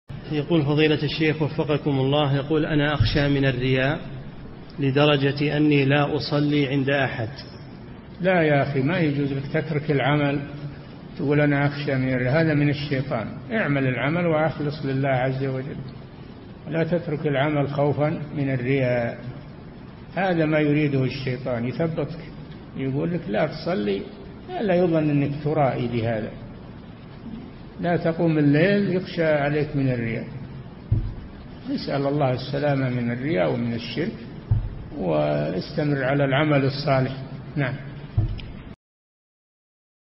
[62 -939] أنا أخشى من الرياء لدرجة أني لا أصلي عند أحد! - الشيخ صالح الفوزان
-[62 -939] السؤال: أنا أخشى من الرياء لدرجة أني لا أصلي عند أحد!
الجواب: لا يا أخي ما يجوز لك تترك العمل وتقول أنا أخشى من الرياء، هذا من الشيطان، اعمل العمل وأخلص لله عز وجل ولا تترك العمل خوفًا من الرياء، هذا ما يريده الشيطان، يثبطك، يقول لك لا تصلِّ لئلا يظن أنك ترائي بهذا، لا تقم الليل يخشى عليك من الرياء، اسأل الله السلامة من الرياء ومن الشرك واستمر على العمل الصالح.
للاشتراك في قناة الشيخ صالح الفوزان

للاستفسار والإقتراحات والدعم
رئيس الفريق
لمشروع كبار العلماء